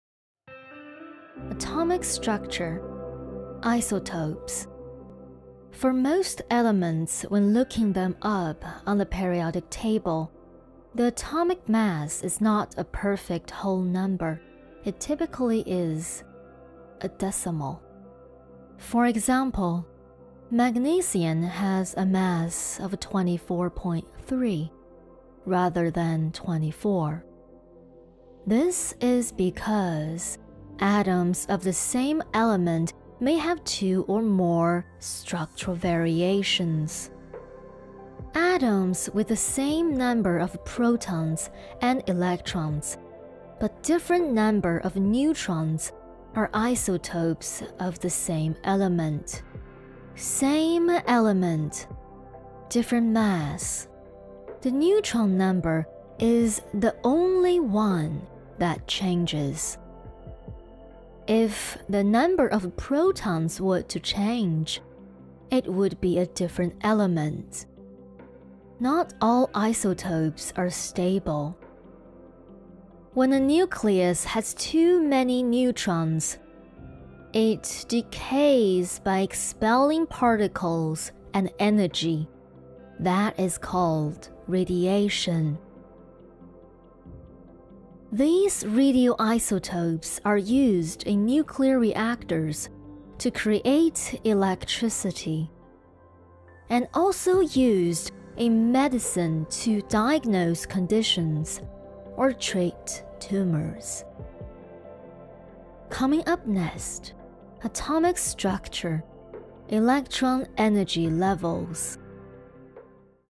Chemistry Basics 1.9 Isotopes｜超简单化学英语基础 1.9 同位素｜化学英语基础入门｜如何学习化学英语｜化学学习｜英语学习
I wrote my notes based on my teacher's in-class PPTs and the school's materials. It helps me to remember what I have learned after I made them into a video. There are in total of 36 videos about general chemistry.

共36篇，内容不是很深，就是一些皮毛。PPT是从我自己的上课笔记和上课用的课本中整理而来的。这套视频主要是帮助自己期末考试复习，没事多听听可能就记住了呀嘻嘻。中文翻译是有道词典来的，可能会有不到位的情况，欢迎大家指出，共同学习提高。

Hey 如果喜欢我的视频，可否点击链接请我喝杯咖啡呢？谢谢你的支持哦！